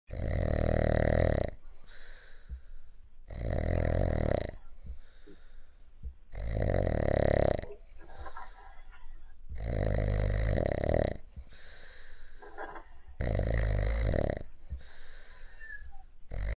🐕😴Pug Peter snoring like he works 9-5 to pay the bills 😅dogs
If you have a snoring dog...